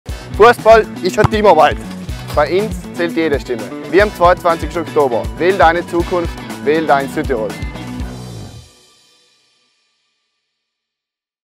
Südtirol wählt, und du?
Fußball ist Teamarbeit. In der Mannschaft zählt jede Stimme. Wie am 22. Oktober bei den Landtagswahlen.